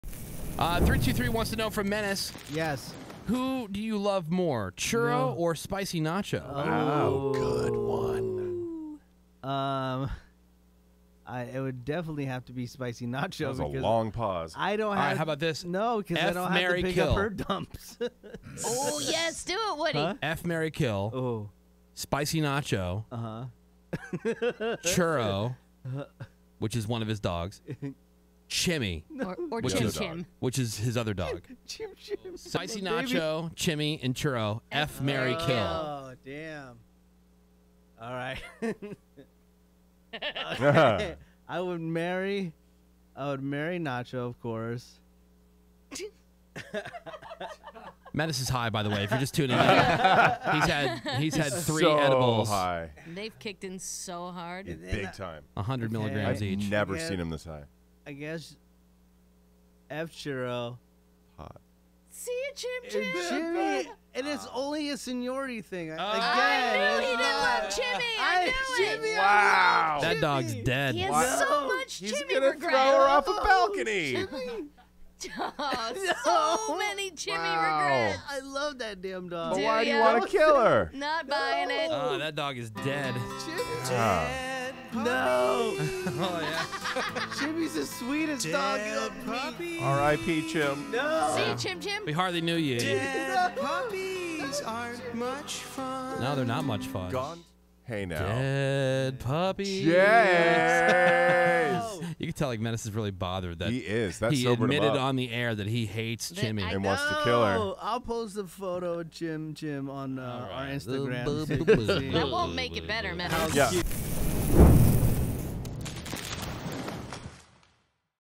EFF, Marry, Kill: Menace Picks Between His Girlfriend & His Dogs
Menace plays EFF, Marry, Kill with his girlfriend and two dogs while HIGH